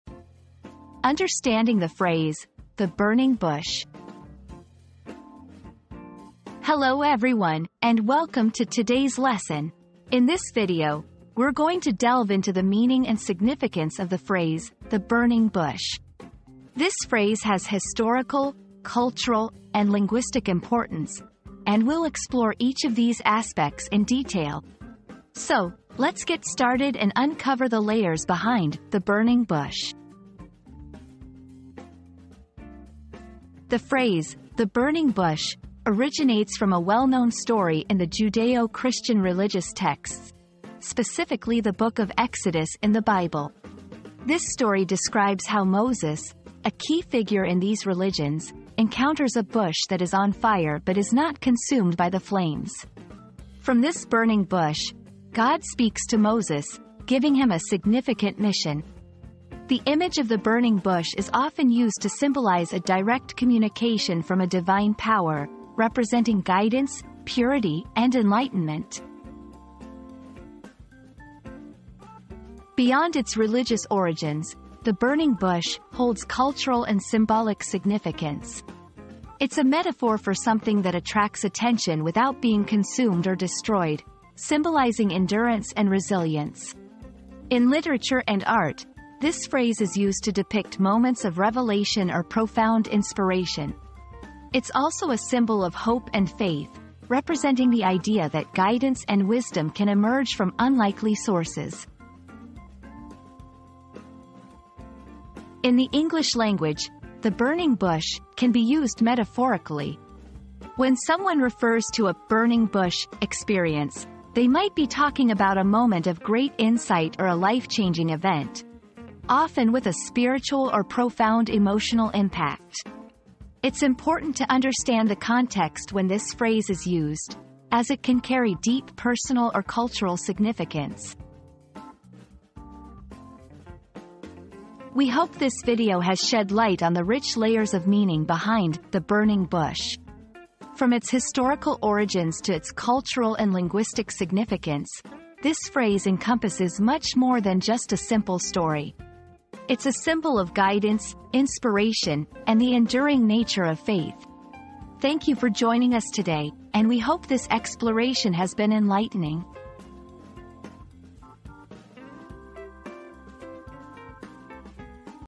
Understanding the Phrase "The Burning Bush"
Unraveling the Mystery: The Burning Bush Explained • Discover the hidden meaning behind the iconic phrase 'The Burning Bush' and its significance in various cultures and religions. Join us on this enlightening journey to unravel the mystery and gain a deeper understanding of this timeless symbol.

00:00 • Introduction - Understanding the Phrase "The Burning Bush"
00:33 • The Origin of "The Burning Bush"
01:12 • Cultural and Symbolic Significance
01:45 • Linguistic Usage